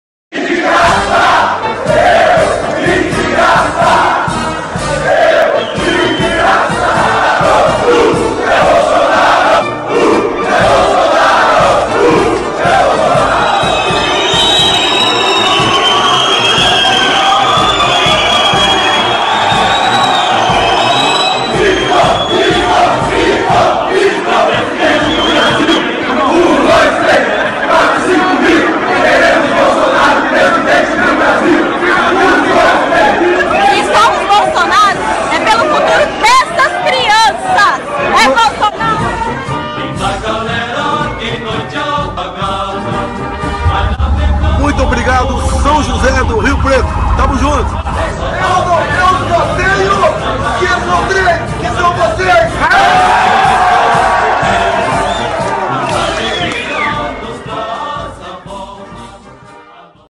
MULTIDÃO RECEBENDO BOLSONARO
Quero ver o Haddad arrastar essa multidão sem comprar o povo com pão com mortadela e R$30...

Desafio o Haddad a tentar fazer qualquer coisa na vida sem trapacear.